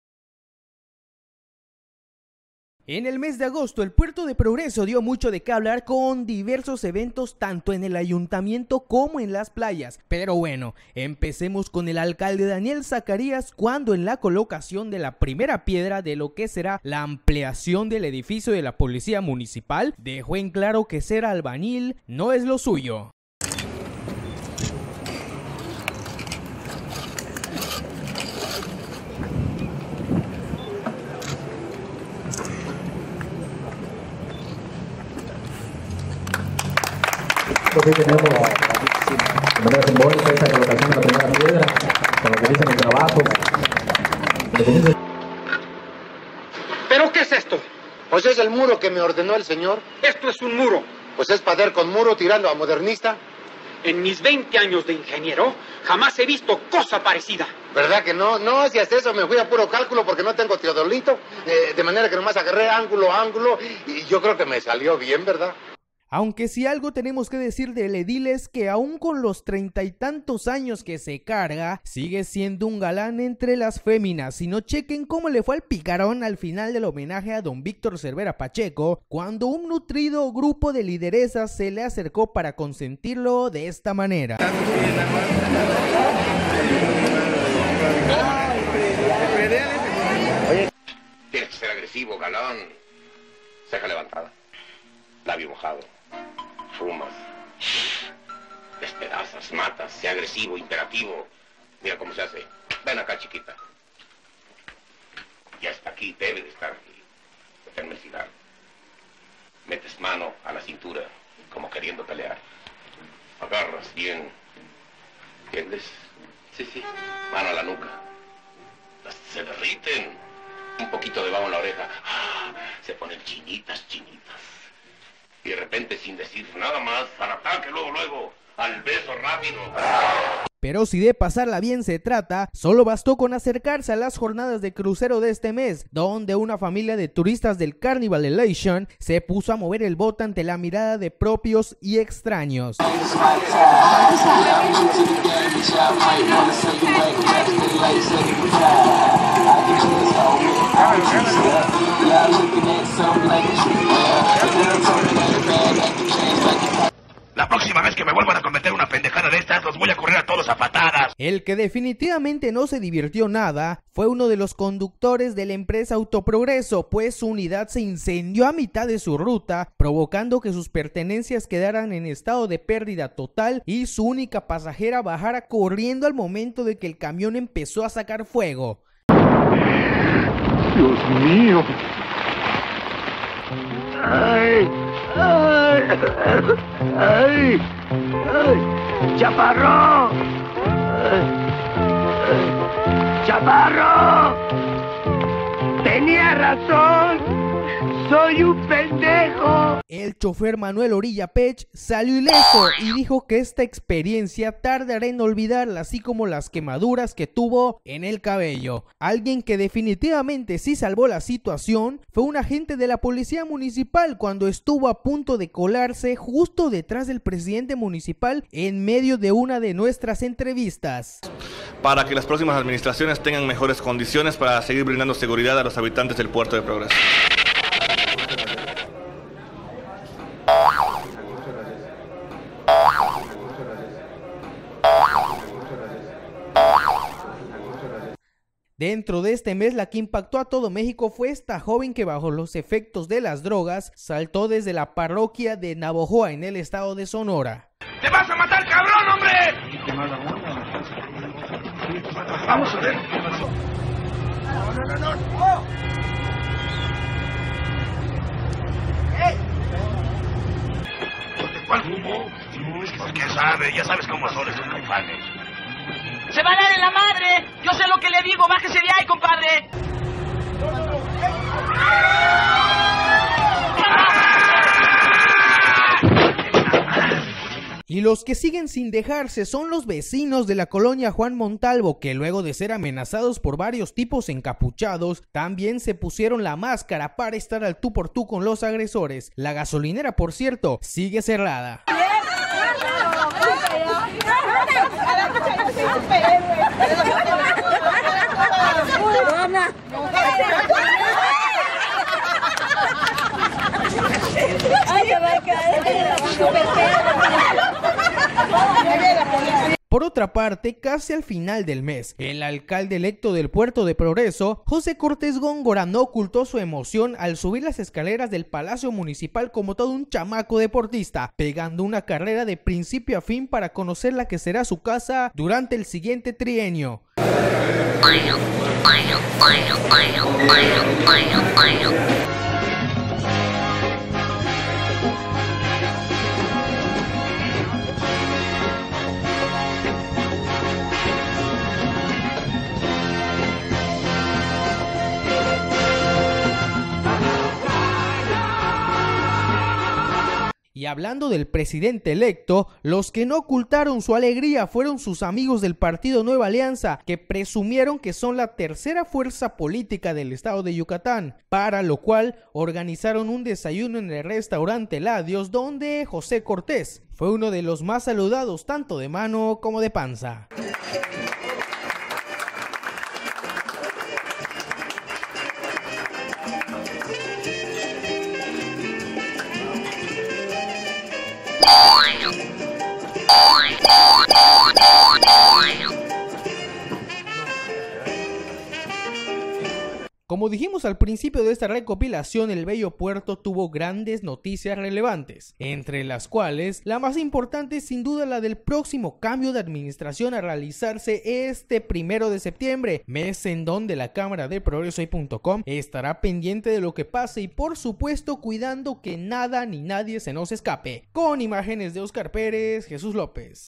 ProgresoHoy.com -- LosResbalones de ProgresoHoy (Agosto 2015)
VÍDEO: ¡ESTRENAMOS NUEVA VÍDEO-SECCIÓN!. Para todos nuestros lectores y con el fin de ofrecerles un rato agradable en torno a lo que vivimos y observamos todos los días en nuestro trabajo informativo o en las noticias del momento, la Redacción de ProgresoHoy les ofrece esta sección con varios aspectos chuscos que esperamos sean de su agrado. Esperamos sus comentarios y sugerencias con el fin de mejorarlo y en breve les pediremos su apoyo para "bautizar" este nuevo espacio.
Puedes ver más vídeos e información en ProgresoHoy.com, el periódico del puerto... LosResbalones de ProgresoHoy, el lado ocurrente de la noticia.